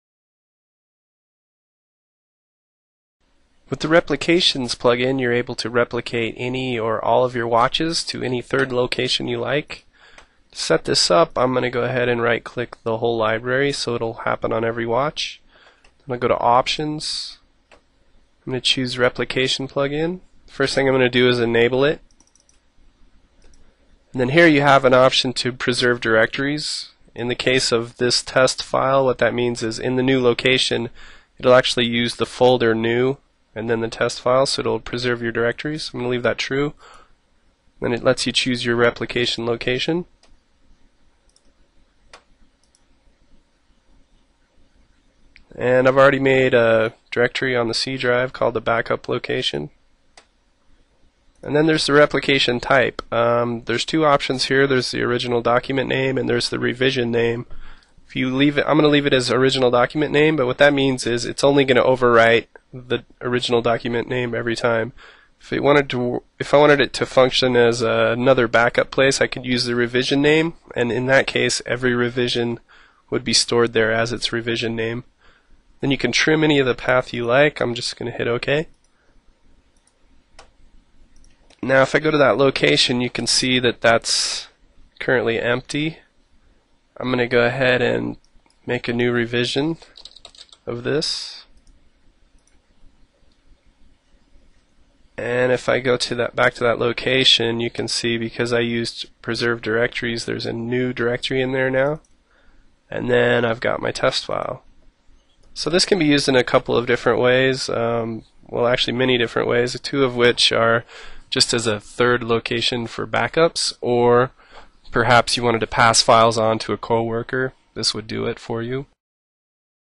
File Hamster - Making Copies with the Replication Plugin
Replicate Files
Many times, in addition to backups of revisions, you also want to keep copies of the current files in separate locations. With the Replication Feature, FileHamster now allows you to keep exact copies of your working versions in a different place.